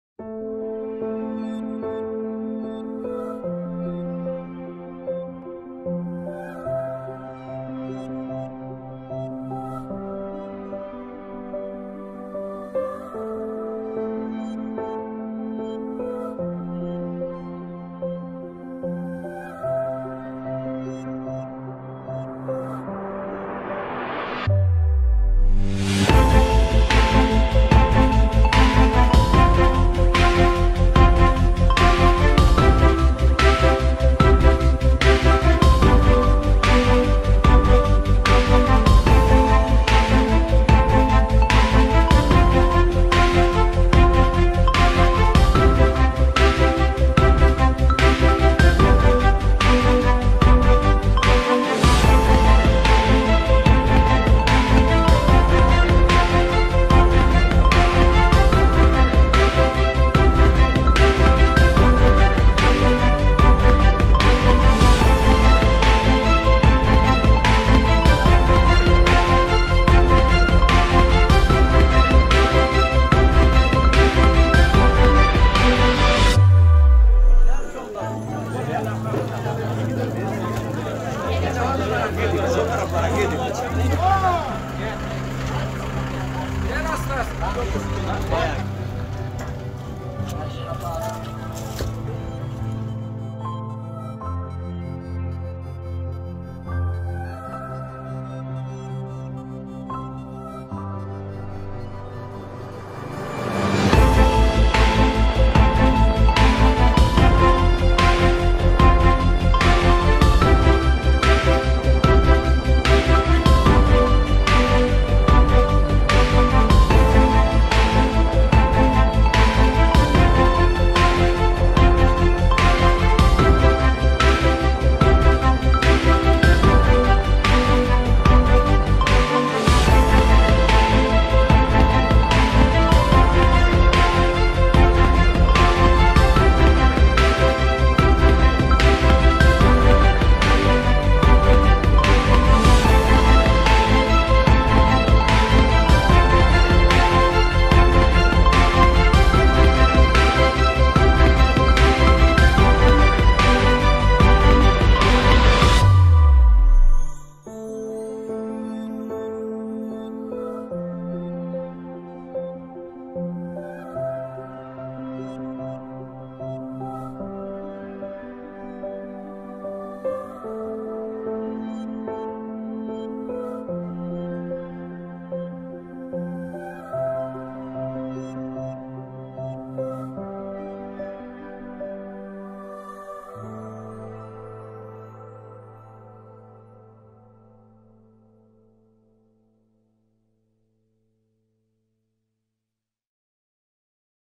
Manavgat drone çekimi. Fidan bağışı organizasyonu
Antalya manavgat için yapılan fidan bağışı organizasyonu havadan drone video çekimleri.

Drone çekimi için önce bizden fiyat teklifi alınız.